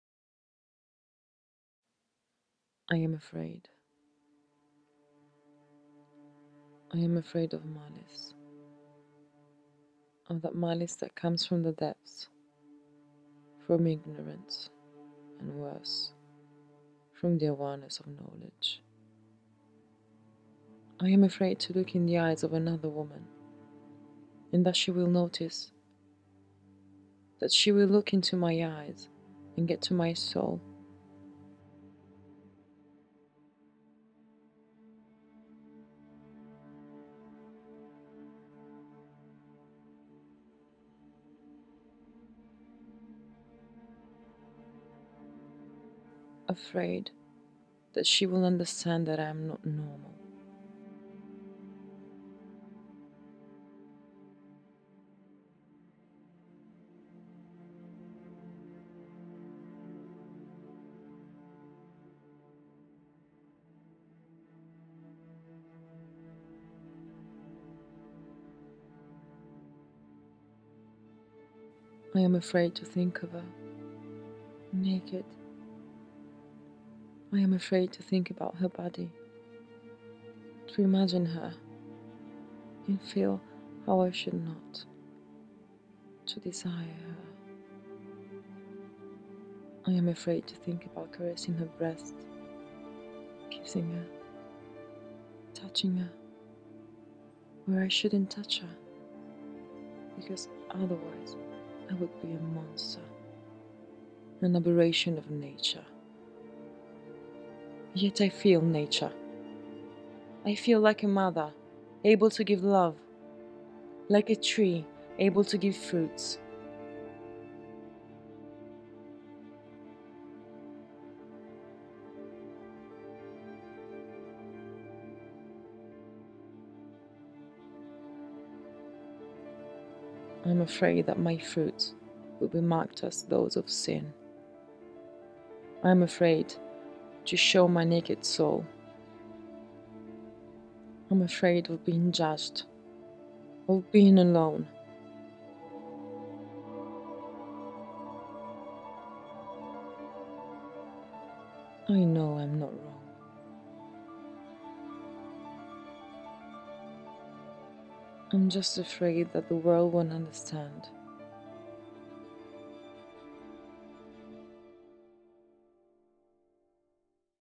Afraid
Un racconto romantico di una giovane bisessuale che insegue il suo sogno d'amore con una bellissima bionda incontrata casualmente nella metropolitana